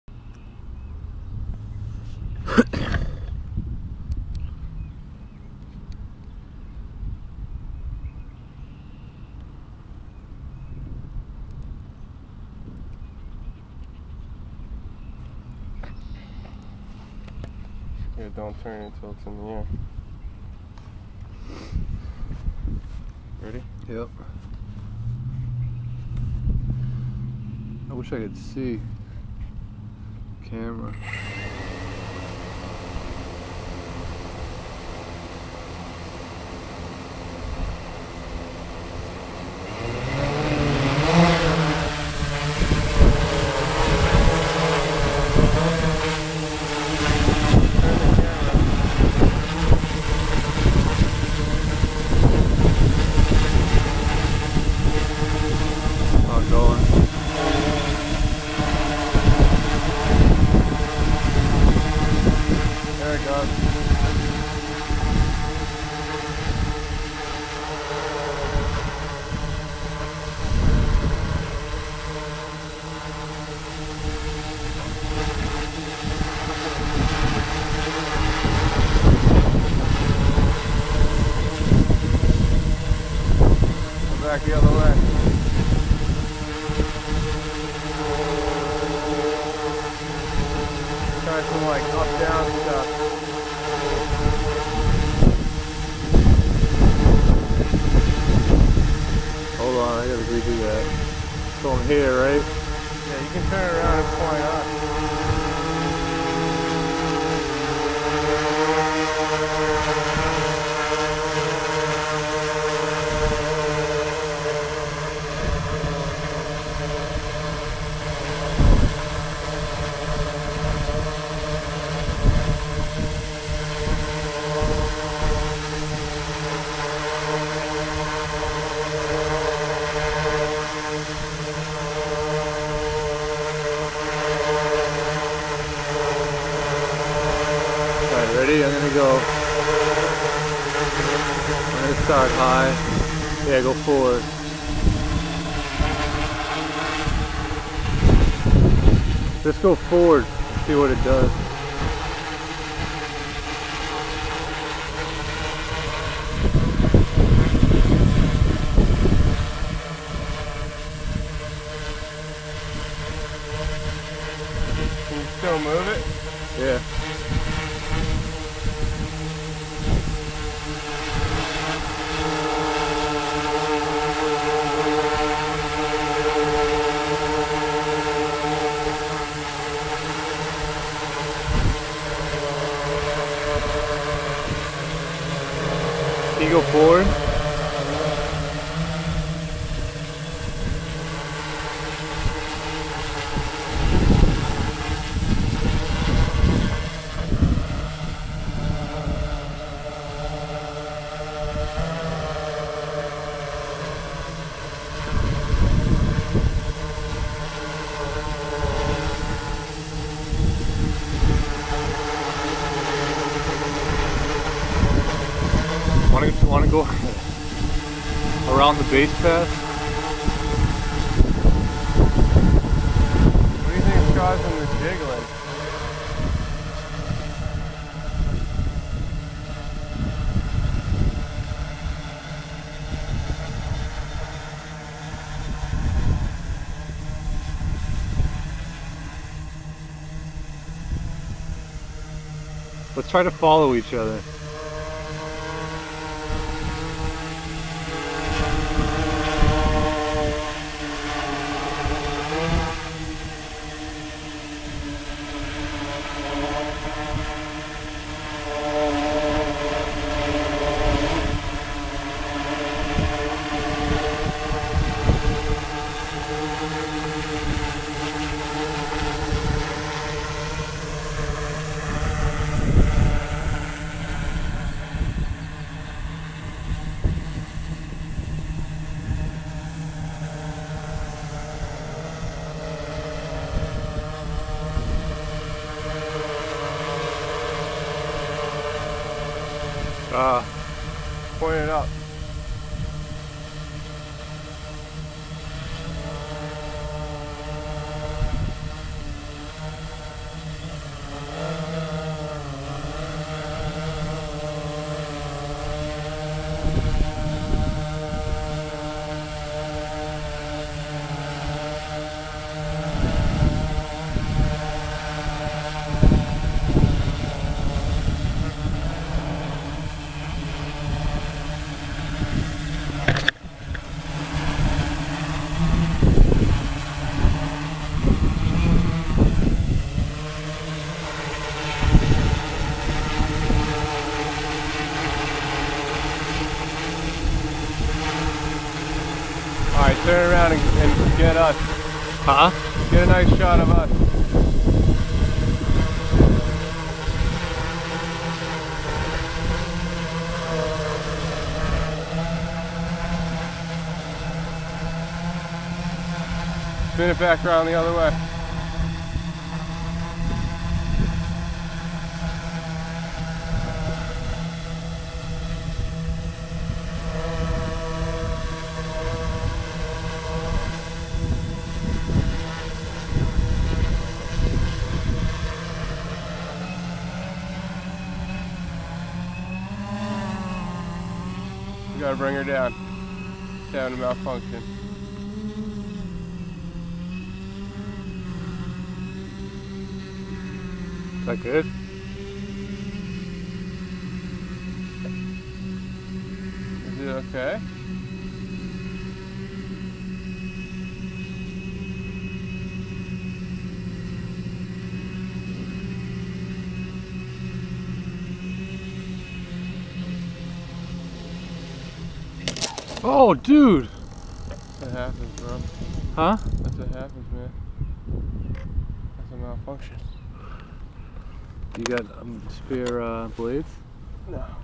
Sky-Hero Spyder6 Trials - Flight 24 Spyder 6 Down again...
ESCs too hot. 40a escs with motors rated for 50a -- picked up 60a

1st Spyder6 to successfully fly and land a DSLR on Youtube

Sky-Hero Spyder 6
Canon 70D
Tamron SP 90mm f/2.8 Di Macro Autofocus
XPS 300X 3 axis gimbal
Wookong
Pulse 16,000 mah
Futaba T10J - Camera
Futaba T8FG - Pilot
T-Motor MN 4120 465kv
T-Motor 60A escs
APC 16x5.5 props
RCCC remote shutter
Immersion RC SpiroNet 5.8GHz Patch Antenna
7" LCD Screen 5.8GHz Diversity System - FatShark / ImmersionRC / DJI
3 - IRC - 5.8 GHz SpiroNET Omni SMA Antenna RHCP
FatShark AttitudeV2 FPV Goggles
FatShark 250mW 5G8 V3 (4S) transmitter FPV

Hexacopter Multiroter S1000 S800 Quadcopter Drone